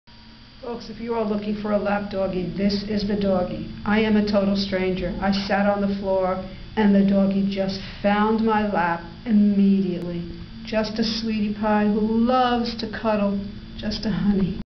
Meet Cupid a Chihuahua Short Coat currently available for adoption at Petango.com! 2/4/2015 7:20:00
Cupid is the perfect name for this ultimate lap doggie. He is super duper cuddly and sweet. The photos and video were taken seconds after I met Cupid. I sat on the floor and he immediately found my lap. Imagine how much love you will get when he knows that you are his forever friend.